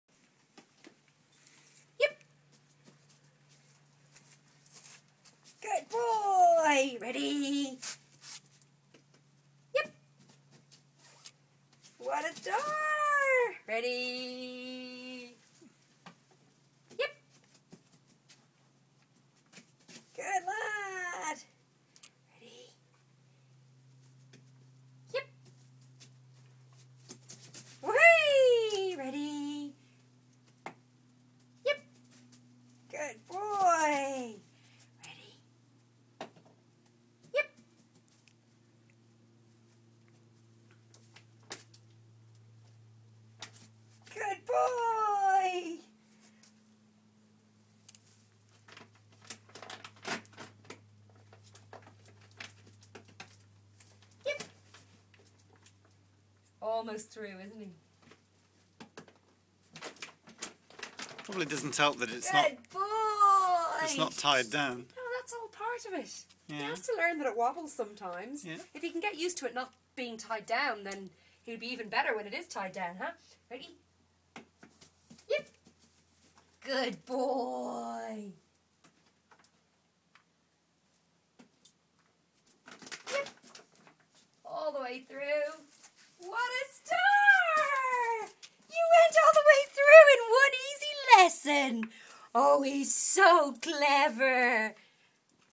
Guinness tunnel training part 2
Guinness at 12 weeks learning how much fun the tunnel can be. Within ten mins, if the right motivation is offered great progress can be made.